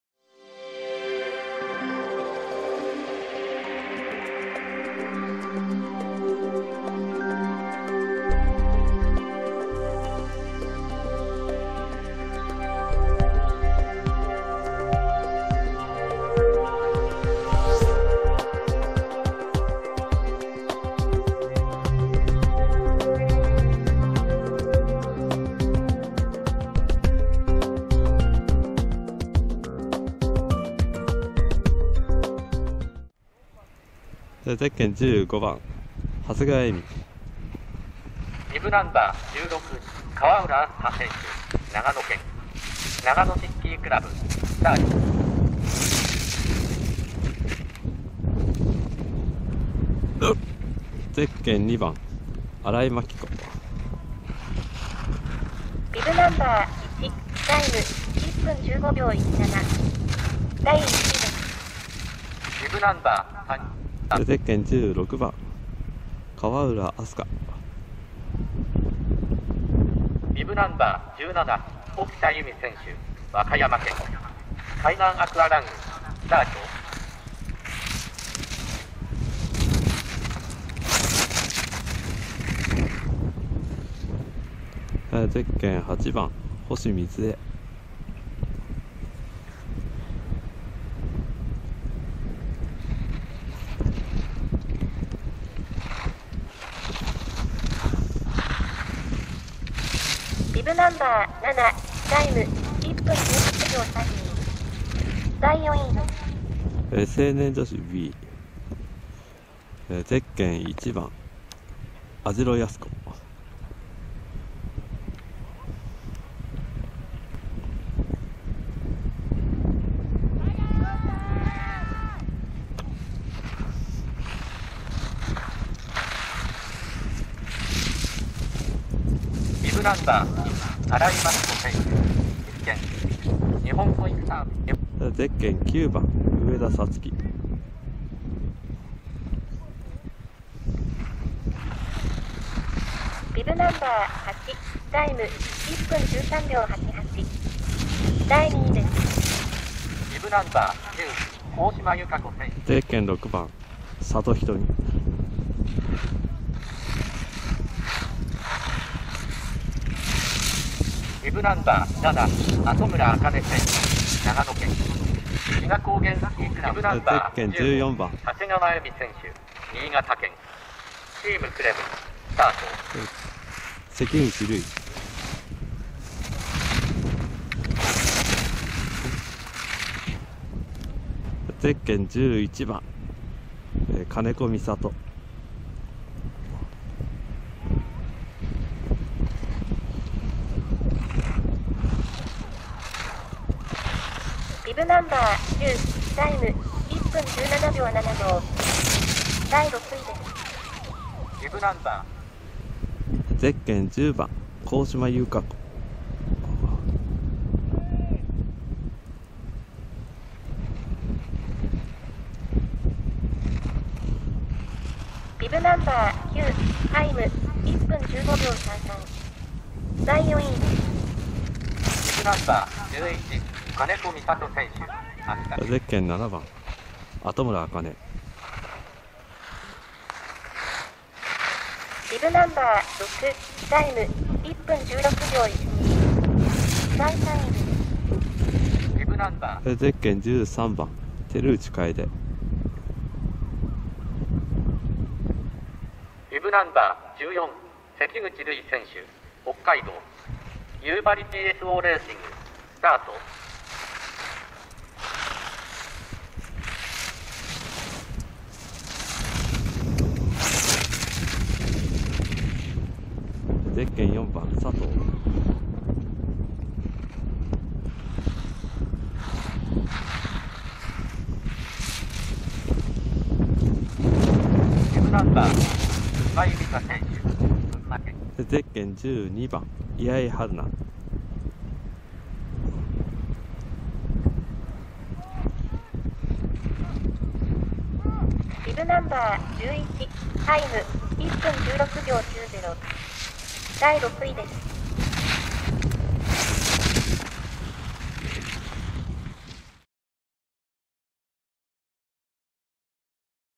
第７４回国民体育大会 スキーアルペン 成年女子Ｂ組 上位入賞者
札幌テイネスキー場で開催された第７４回スキー国体アルペン競技の成年女子Ｂ組上位入賞者　※１３位の宮木選手は撮りそびれた！

長谷川絵美　新井真季子　川浦あすか　星瑞枝　網代慈子　上田皐
佐渡仁美　関口留依　金子未里　神嶋由佳子　後村茜　佐藤千紗
居相はるな